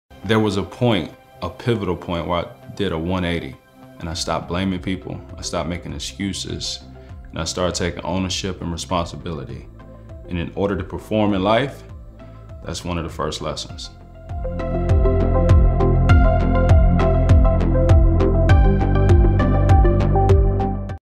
"Ownership" The IF in Life by Rashad Jennings
“Whether you realize it or not, certain moments in life, no matter how significant or small they seem, can change your path and have a positive impact on where you go and how successful you become. The IF in Life is about those “IF” moments that have shaped the man I’ve become and brought about the realization of my dreams. My hope for this book is that it brings motivation and inspires young people to become their best self.” —Rashad Jennings